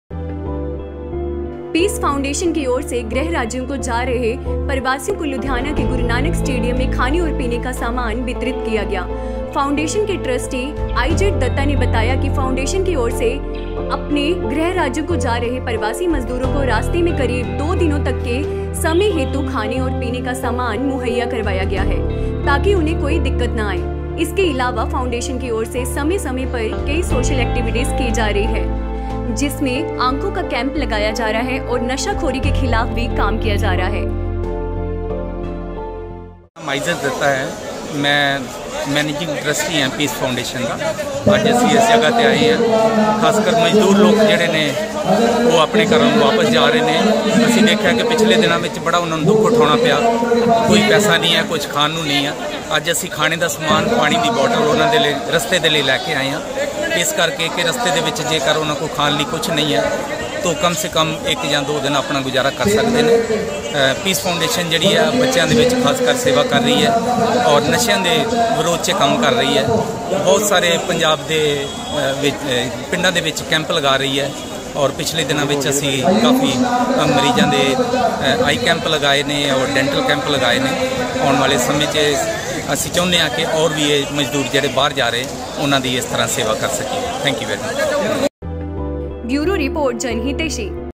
पीस फाउंडेशन की ओर से गुरू नानक स्टेडियम में खाने और पीने का सामान वितरित किया गया।
पीस फाउंडेशन की ओर से गृह राज्यों को जा रहे प्रवासियों को लुधियाना के गुरू नानक स्टेडियम में खाने और पीने का सामान वितरित किया गया।